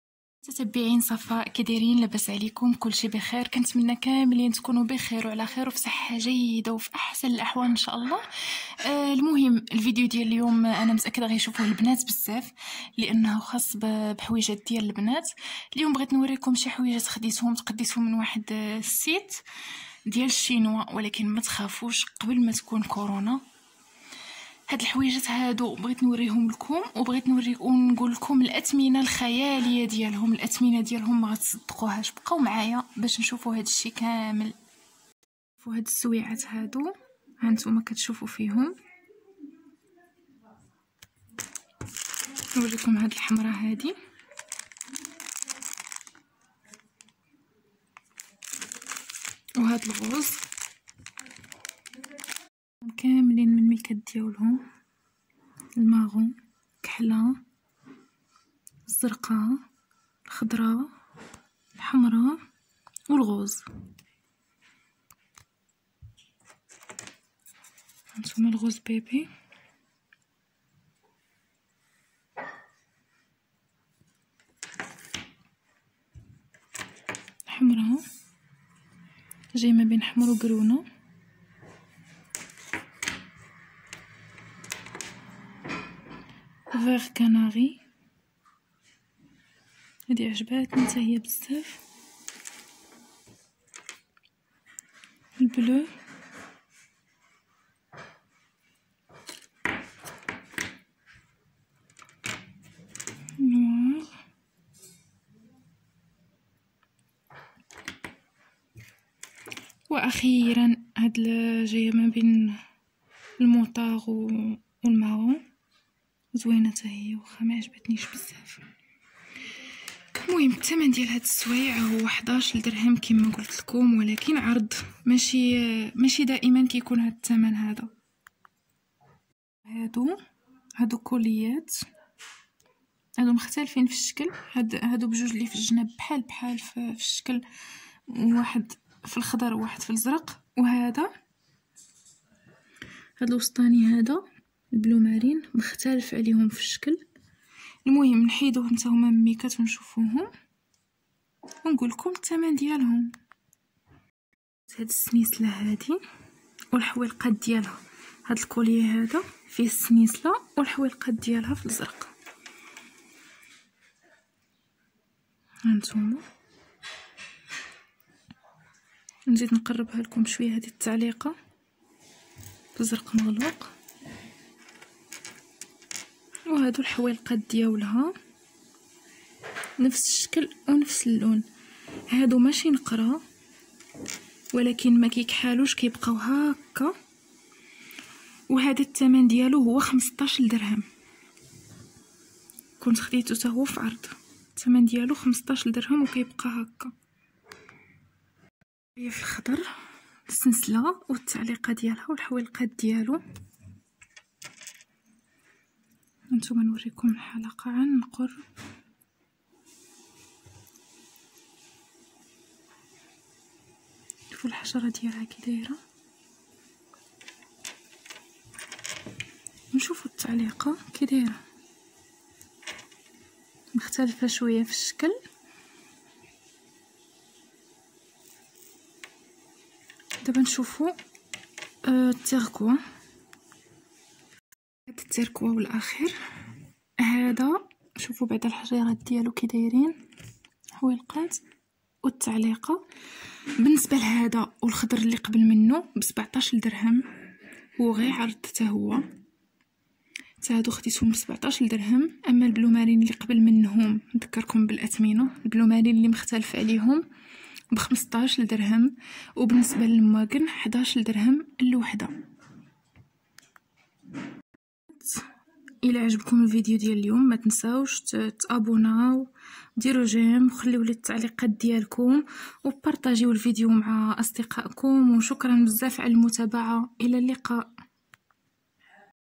مشترياتي اكسيسوارات من ALIEXPRESS بثمن خيالي 😲🛍💍💎
ساعات يدويه وقلادات ودبابيس وغير ذلك باثمنه لن تصدق